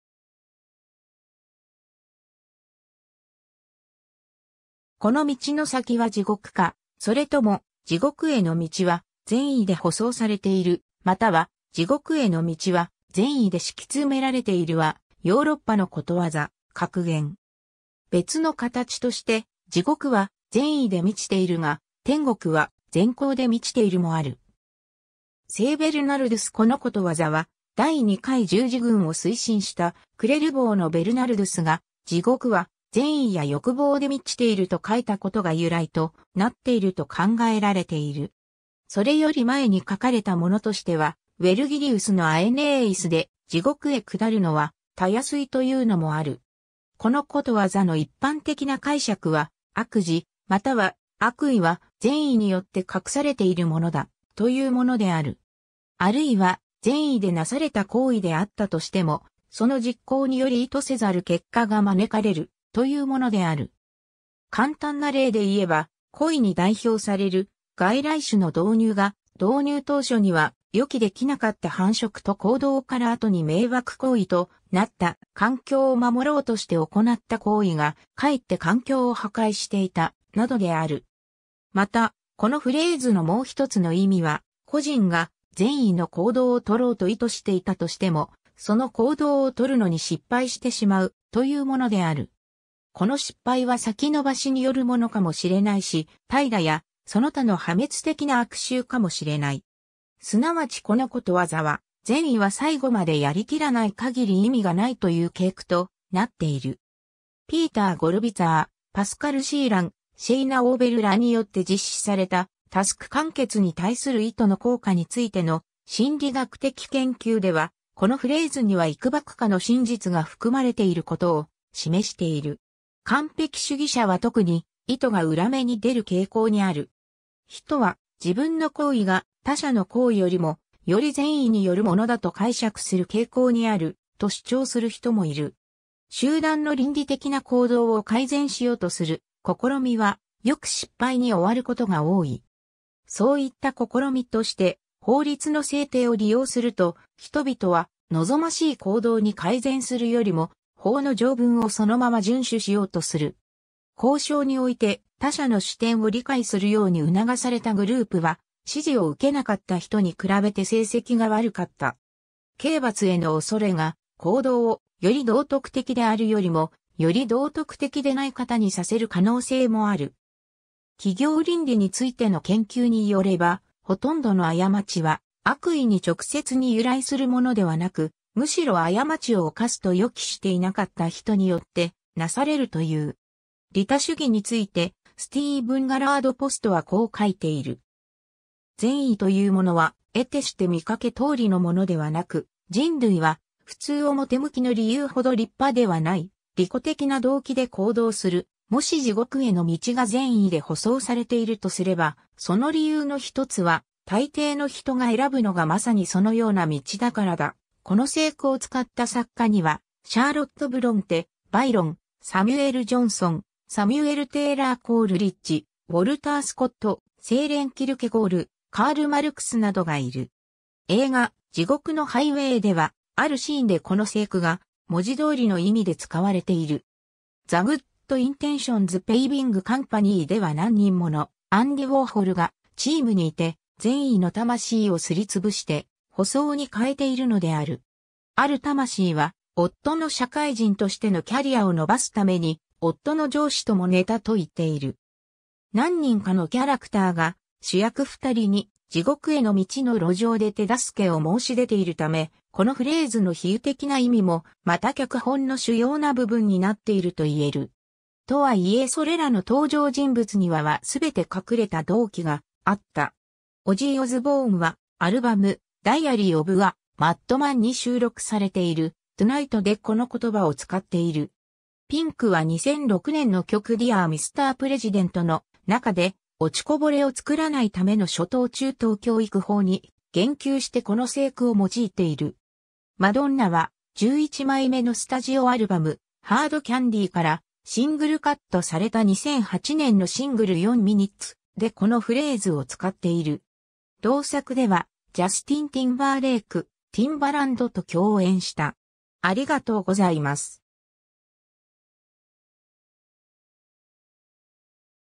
地獄への道は善意で舗装されている
この道の先は地獄か、それとも…… 「地獄への道は善意で舗装されている」（じごくへのみちはぜんいでほそうされている）、または「地獄への道は善意で敷き詰められている」（じごくへのみちはぜんいでしきつめられている、英: The road to hell is paved with good intentions）はヨーロッパのことわざ、格言。
別の形として、「地獄は善意で満ちているが、天国は善行で満ちている」（じごくはぜんいでみちているが、てんごくはぜんこうでみちている）（英: Hell is full of good meanings, but heaven is full of good works）もある。
 聖ベルナルドゥス このことわざは、第2回十字軍を推進したクレルヴォーのベルナルドゥスが「地獄は善意や欲望で満ちている」("L'enfer est plein de bonnes volontés ou désirs") と書いた(1150年ごろ)ことが由来となっていると考えられている。
それより前に書かれたものとしては、ウェルギリウスのアエネーイスで「地獄へ下るのはたやすい」("facilis descensus Averno") というのもある。
 このことわざの一般的な解釈は、「悪事または悪意は、善意によって隠されているものだ」、というものである。
あるいは「善意でなされた行為であったとしても、その実行により意図せざる結果が招かれる」、というものである。
簡単な例でいえば、コイに代表される外来種の導入が、導入当初には予期できなかった繁殖と行動から後に迷惑行為となった、環境を守ろうとして行った行為が、かえって環境を破壊していた、などである。
 また、このフレーズのもう一つの意味は「個人が善意の行動を取ろうと意図していたとしても、その行動を取るのに失敗してしまう」、というものである。
この失敗は先延ばしによるものかもしれないし、怠惰やその他の破滅的な悪習かもしれない。
すなわちこのことわざは、善意は最後までやりきらない限り意味がないという警句となっている。
 ピーター・ゴルヴィツァー(Peter Gollwitzer)、パスカル・シーラン(Paschal Sheeran)、シェイナ・オーベル(Sheina Orbell)らによって実施された、タスク完結に対する意図の効果についての心理学的研究では、このフレーズにはいくばくかの真実が含まれていることを示している。
完璧主義者は特に、意図が裏目に出る傾向にある。
人は、自分の行為が他者の行為よりも、より善意によるものだと解釈する傾向にある、と主張する人もいる。
 集団の倫理的な行動を改善しようとする試みは、よく失敗に終わることが多い。
そういった試みとして法律の制定を利用すると、人々は望ましい行動に改善するよりも、法の条文をそのまま遵守しようとする。
交渉において、他者の視点を理解するように促されたグループは、指示を受けなかった人に比べて成績が悪かった。
刑罰への恐れが、行動をより道徳的であるよりも、より道徳的でない方にさせる可能性もある。
企業倫理についての研究によれば、ほとんどの誤ちは、悪意に直接に由来するものではなく、むしろ過ちを犯すと予期していなかった人によってなされるという。
 利他主義についてスティーブン・ガラード・ポストはこう書いている。
善意というものは、えてして見かけ通りのものではなく、人類はふつう表向きの理由ほど立派ではない、利己的な動機で行動する――「もし地獄への道が善意で舗装されているとすれば、その理由の一つは、大抵の人が選ぶのがまさにそのような道だからだ」 この成句を使った作家には、シャーロット・ブロンテ、バイロン、サミュエル・ジョンソン 、サミュエル・テイラー・コールリッジ、ウォルター・スコット 、セーレン・キェルケゴール、カール・マルクスなどがいる。
 映画『地獄のハイウェイ』では、あるシーンでこの成句が文字通りの意味で使われている。
“The Good Intentions Paving Company” (善意舗装会社)では何人ものアンディ・ウォーホルがチームにいて、善意の魂をすり潰して舗装に替えているのである。
ある魂は「夫の社会人としてのキャリアを伸ばすために夫の上司とも寝た」と言っている。
何人かのキャラクターが主役ふたりに「地獄への道」の路上で手助けを申し出ているため、このフレーズの比喩的な意味もまた脚本の...